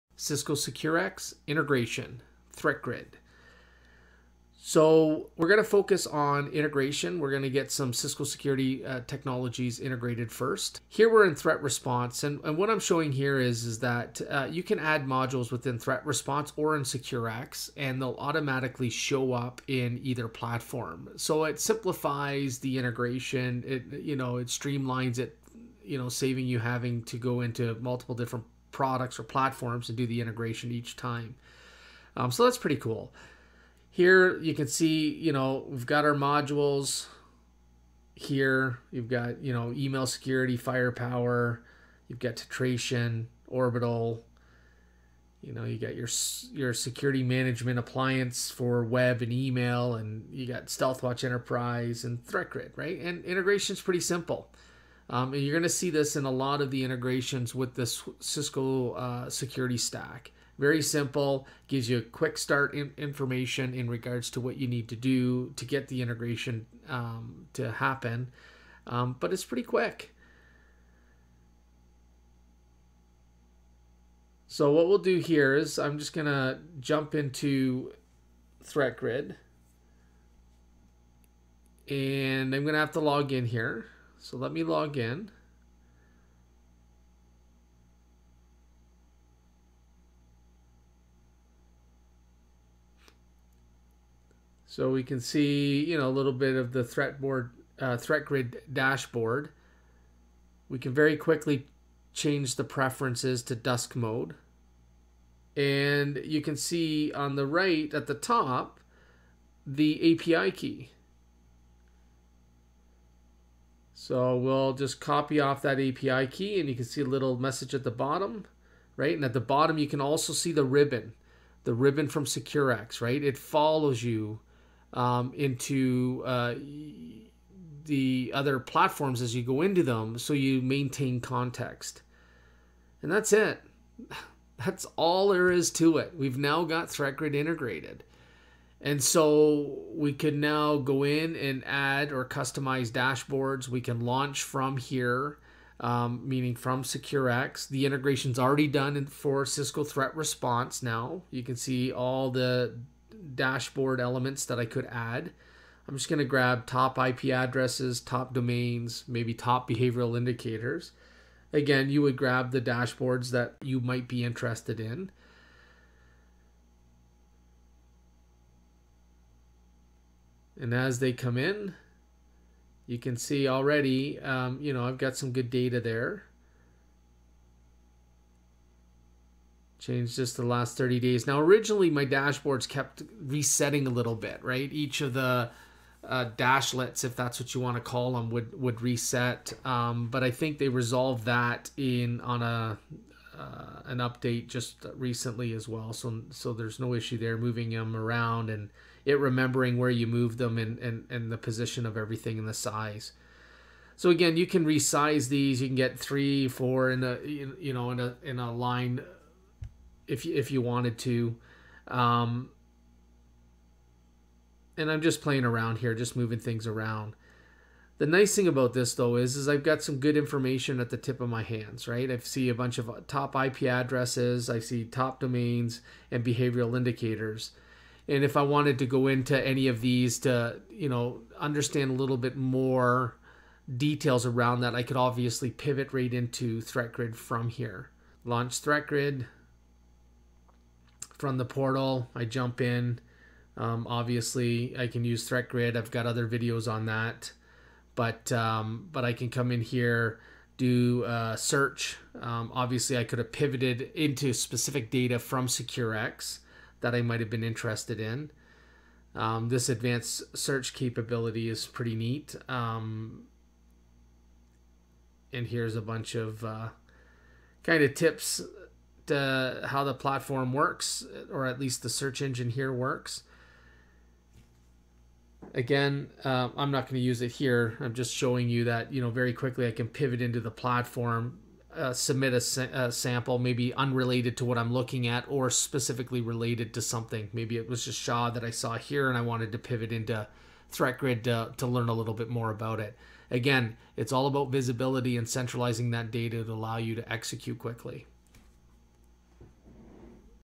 Cisco SecureX: Integration Cisco Threatgrid
In this video we will be integrating Cisco Threatgrid with Cisco SecureX

More on SecureX: Connect Cisco's integrated security portfolio to your existing infrastructure for a consistent experience that unifies visibility, enables automation, and strengthens your security across network, endpoints, cloud, and applications. In this video we will introduce SecureX and do a demo walk through.